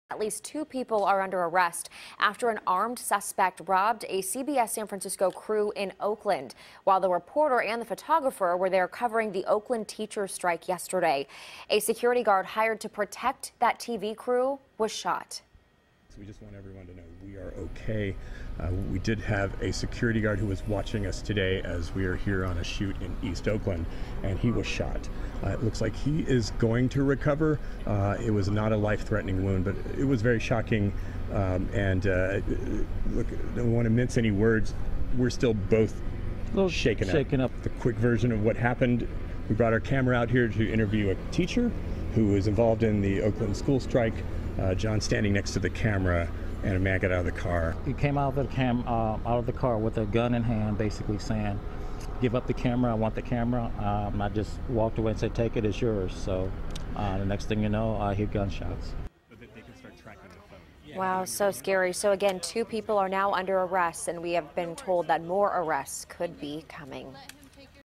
Reporter News Crew Robbed Shootout with Security Guard Maurice Turner Donald Jeter Lolita Brinson
Maurice Turner, Donald Jeter, Lolita Brinson, Matt Meredith